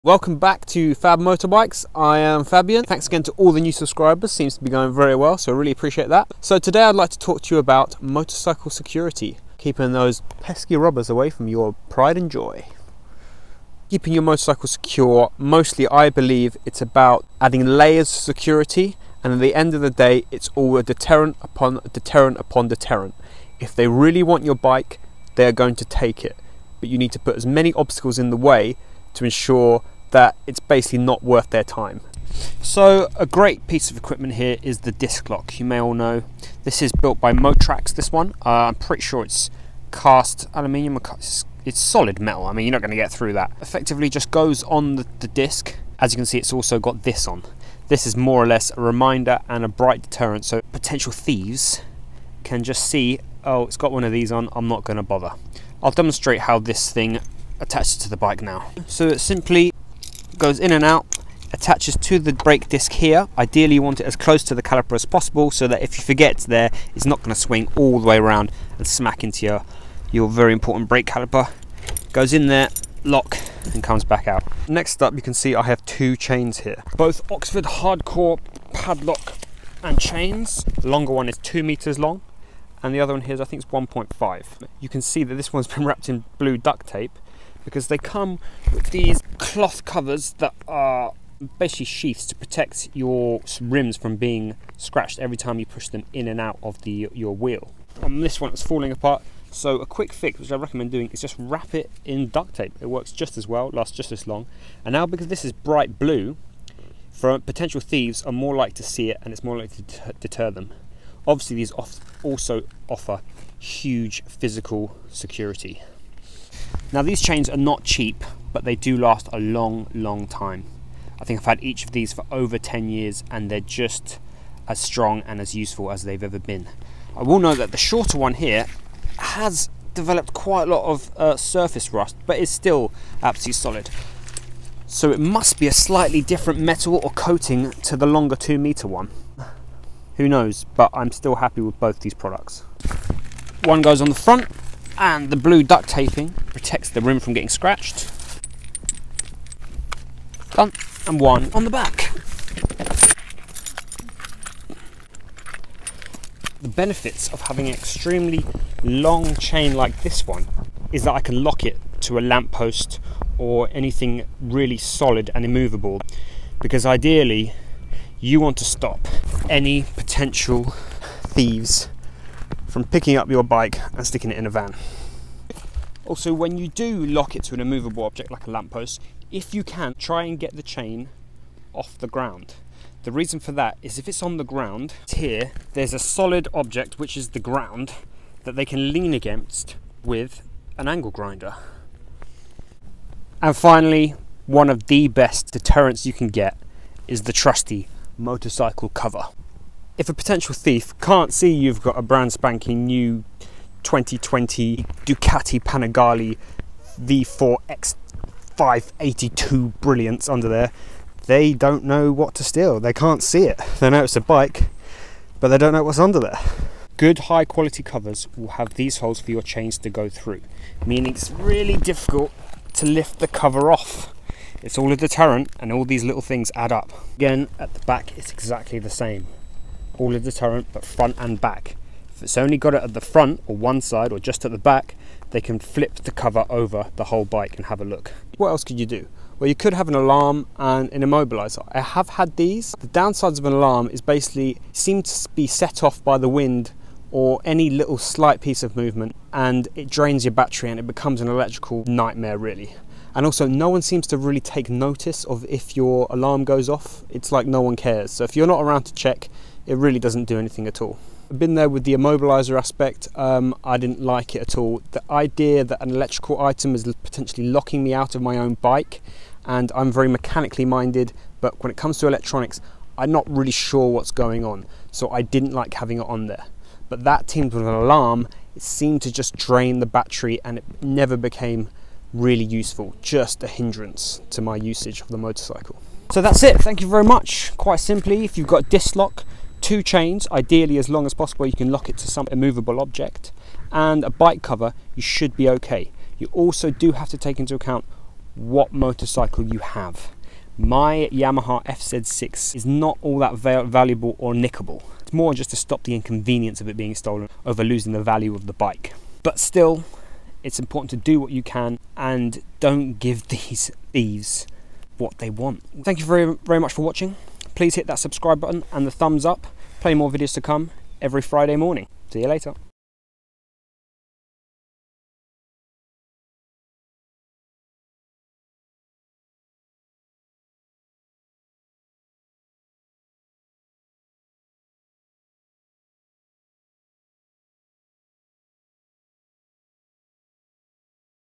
Motorcycle security | How to lock up your bike and protect it from thieves | Lockdown that motorbike
Motorcycle security | Locking up your bike and protecting it from thieves | Lockdown that motorbike

Get your 2m oxford chain:

Motorcycle Security is so important. Keeping you bike locked and any from thieves is essential. Lock is securely with the tips given here. keep you bike locked down